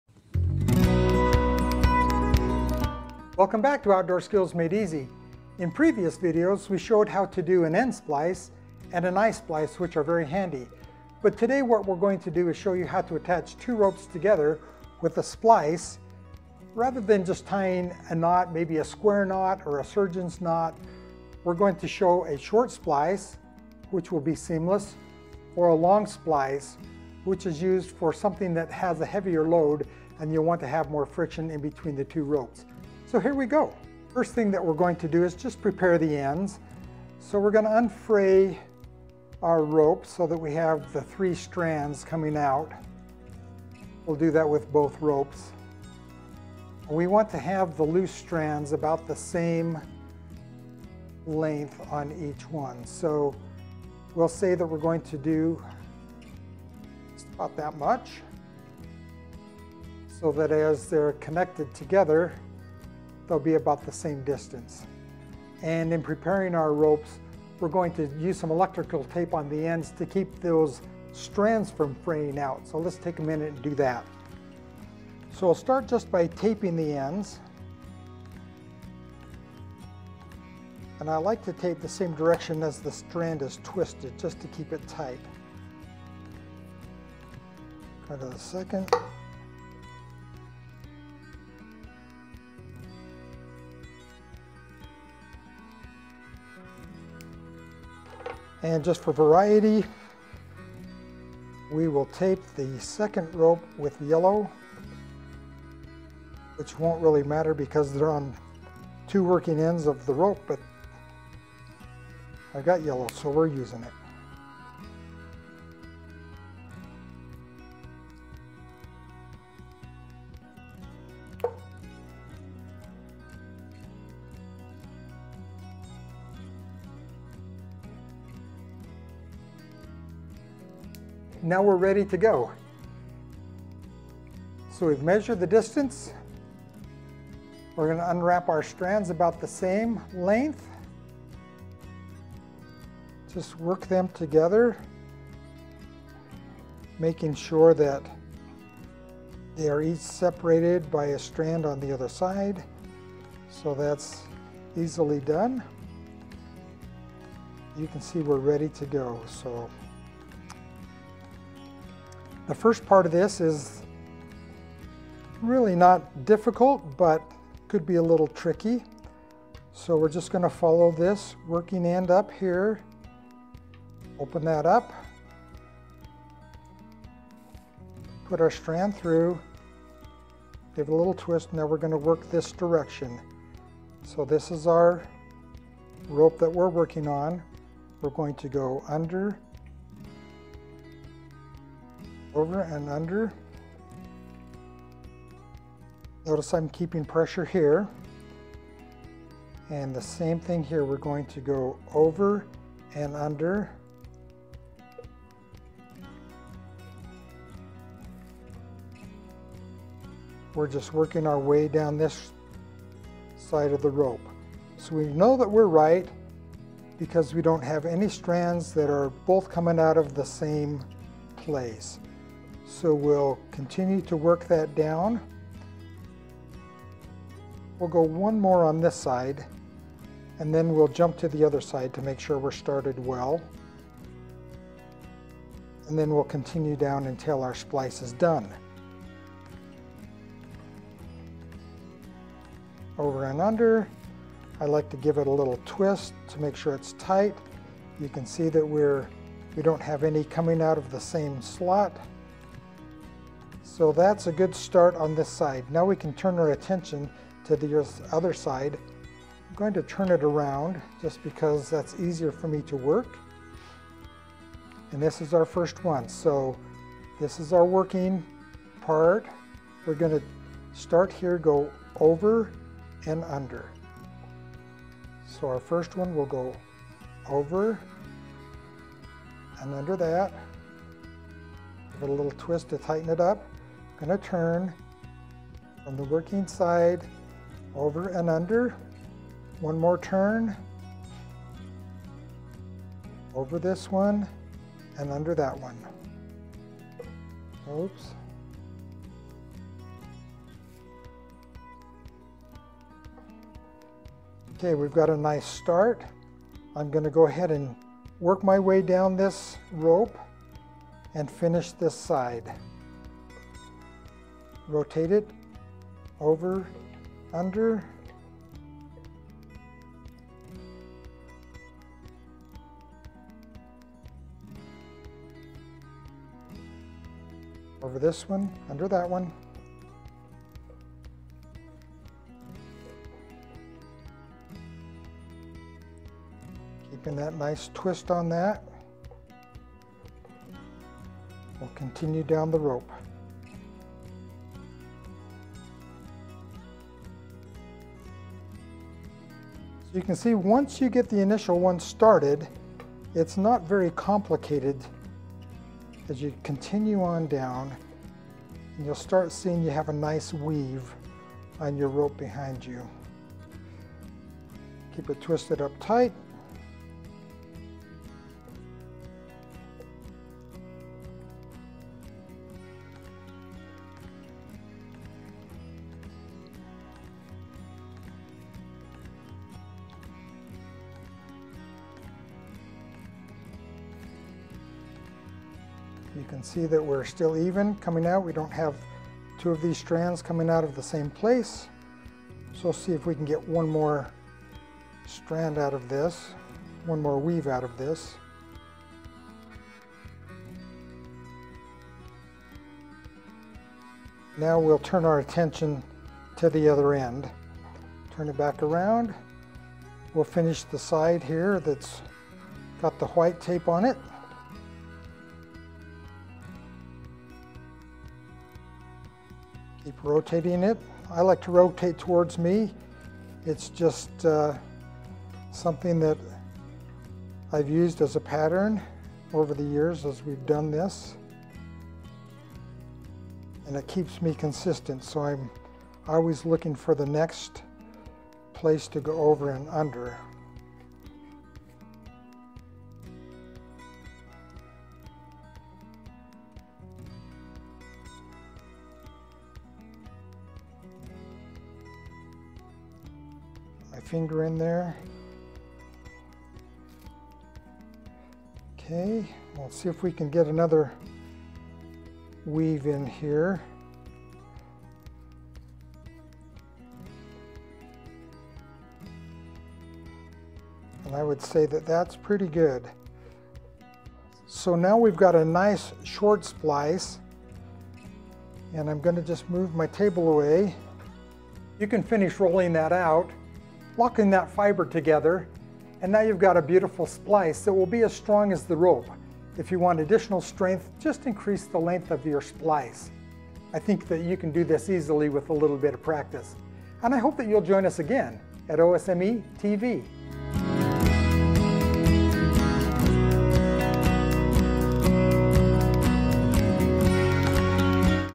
How to Splice Ropes Together | Rope Splicing | Connecting Ropes
Today we're going to show you how to splice ropes together. Rope splicing is fairly easy, so stick around and we'll show you how to connect ropes without knots!

Chapters
0:00 Intro
0:33 Prepare our rope
2:11 Let's go!
8:00 Finish up
8:13 Outro